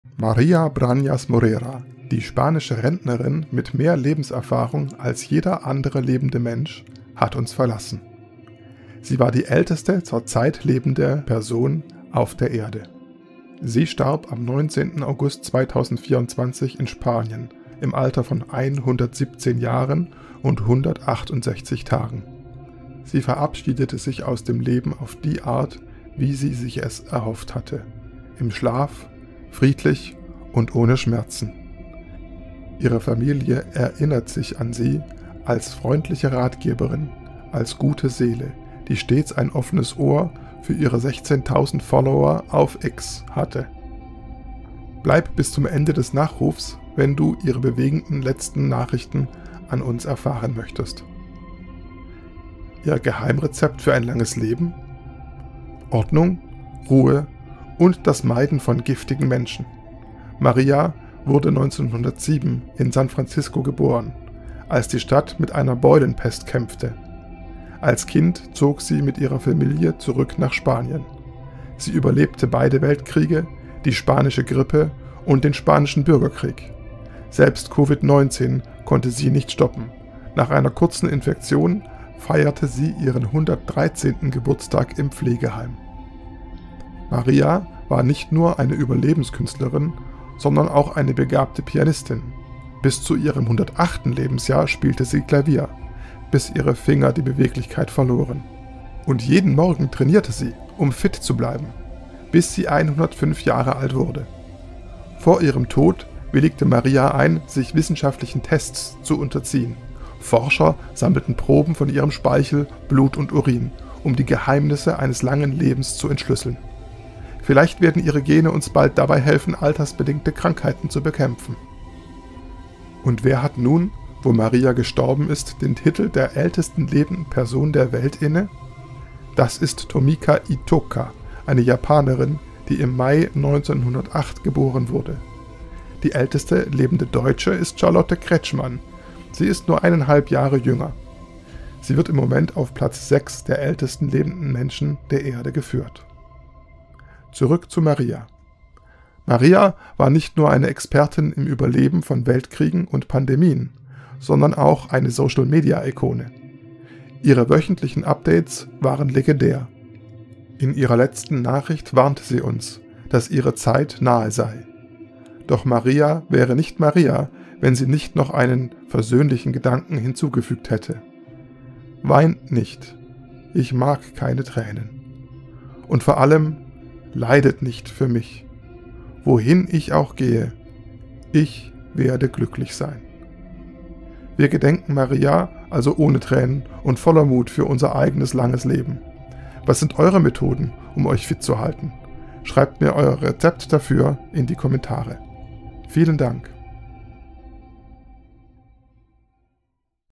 Ältester Mensch gestorben! Ihre letzte Nachricht ist bewegend!
Maria Branyas Morera war die älteste lebende Person auf der Erde.

Maria Branyas Morera war eine spanische Altersrekordlerin. Nach dem Tod der 118-jährigen französischen Ordensfrau Lucile Randon am 17. Januar 2023 war sie der älteste lebende Mensch der Welt, dessen Alter verifiziert wurde. Am 21. April 2023 übertraf sie das Alter von Ana Vela Rubio und wurde damit zur ältesten Person, die jemals in Spanien lebte. Nach dem Tod der Japanerin Fusa Tatsumi wurde sie zur letzten noch lebenden Person, die im Jahr 1907 geboren war. Als sie am 9. Mai 2024 das Alter von Delphia Welford überschritt, stieg sie in die Top 10 der ältesten Menschen ein.